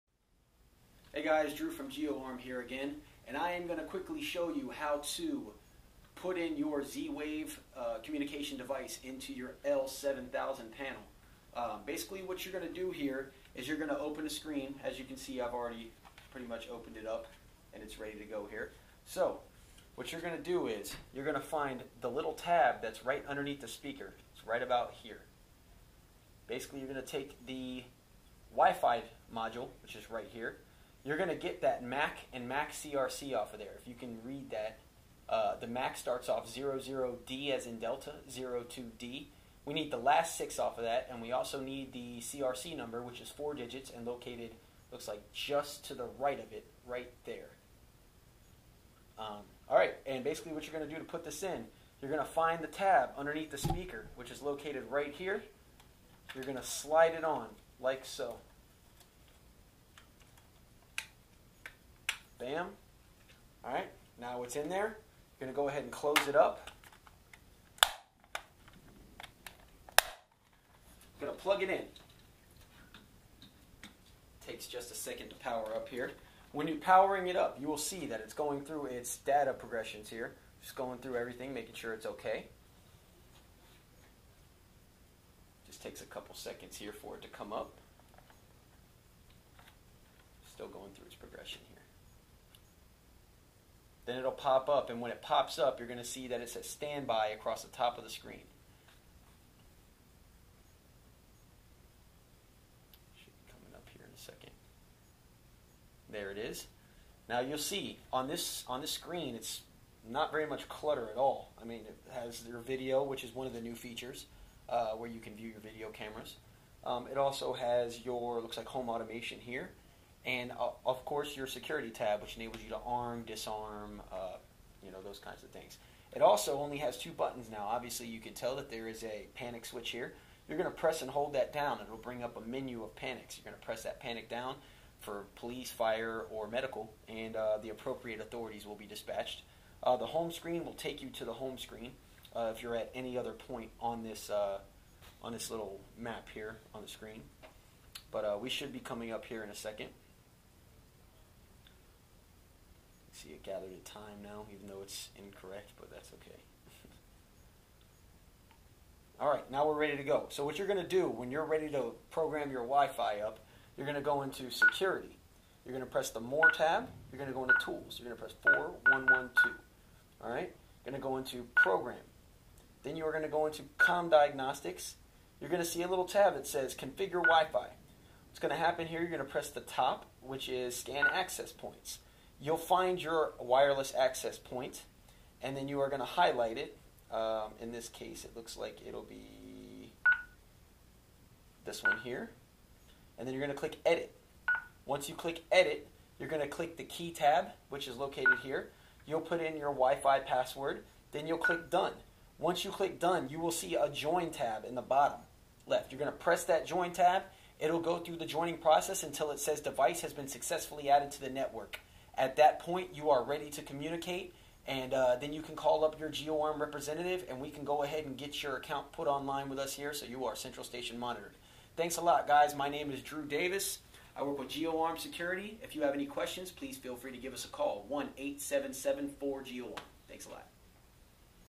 How to Install and Configure Wifi in Honeywell L7000
A brief overview of installing and configuring your Honeywell WiFi Communication device into your Honeywell L7000 Security System. Send L7000 signals over the WiFi local area network with the L5100-WIFI. Pay Less for DIY Security, Go GEOARM.